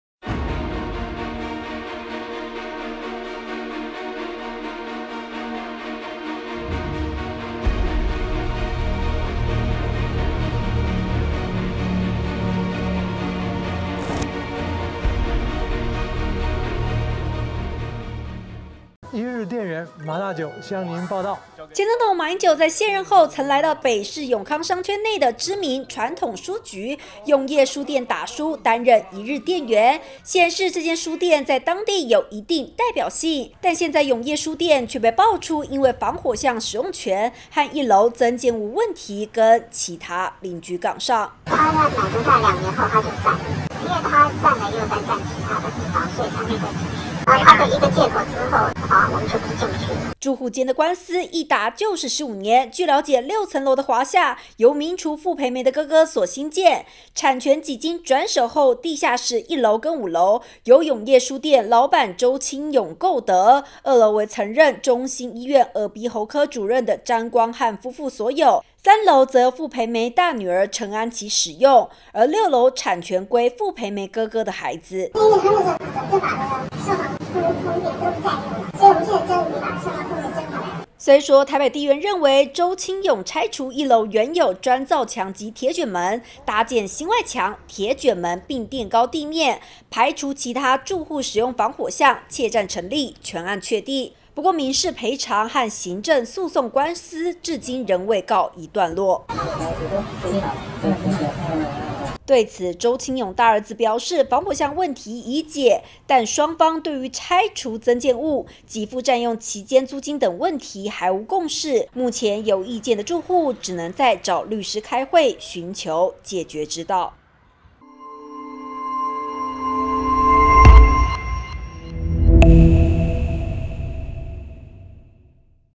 CTWANT 社會傳真 永康商圈名書店惹惱鄰居 傅培梅家族提告纏訟15年
提起台北市的永康商圈，大家想到的是包括鼎泰豐在內的各具特色餐廳美食，但很多人不知道，名廚傅培梅的烹飪教室就曾開在永康街裡，現在由大女兒程安琪接棒，繼續傳承美食文化，至於傅培梅烹飪教室樓下的「永業書局」，則是知名的在地書店，前總統馬英九曾到這家書店擔任一日店員，顯示「永業書局」的「江湖地位」，不料「永業書局」和樓上鄰居因為1樓防火巷使用權和增建物等問題，打了15年官司，至今未歇。